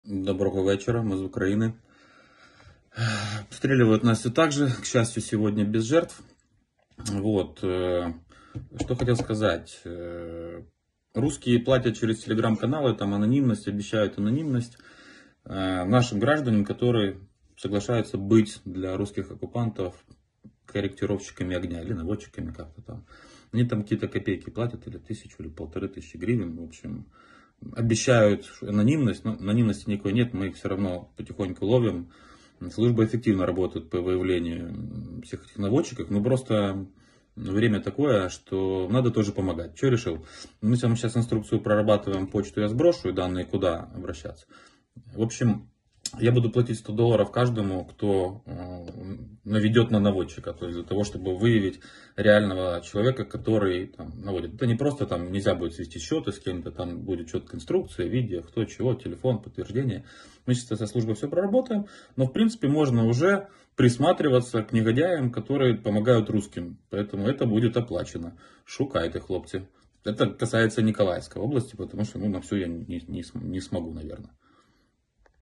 На Миколаївщині обіцяють платити по 100 доларів за інформацію про коригувальників ворожого вогню
Голова Миколаївської ОВА Віталій Кім про ситуацію в місті та області

Джерело: телеграм-канал Голови Миколаївської ОВА Віталія Кіма